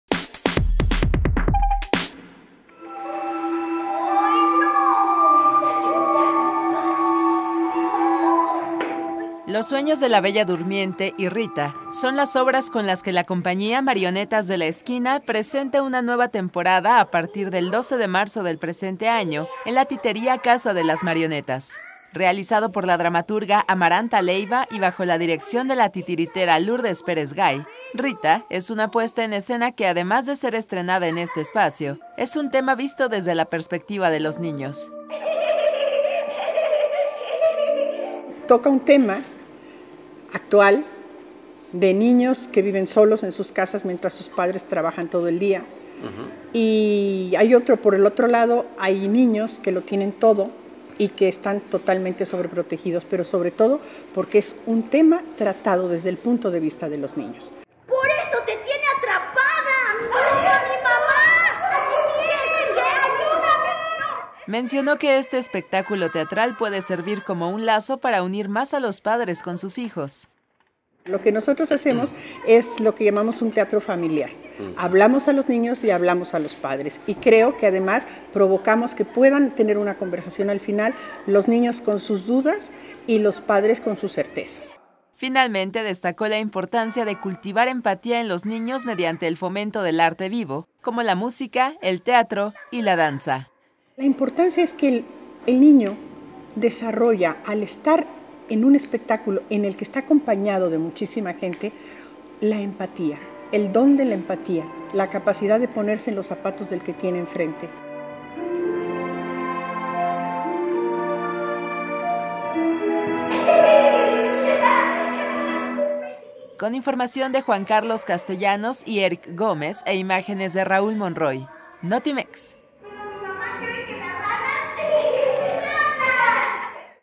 La Titería Casa de las Marionetas, lugar de arte y tradición titiritera para las familias
"Los sueños de la Bella Durmiente" y "Rita" son las obras con las que la compañía "Marionetas de la esquina" presenta una nueva temporada en La Titería Casa de las Marionetas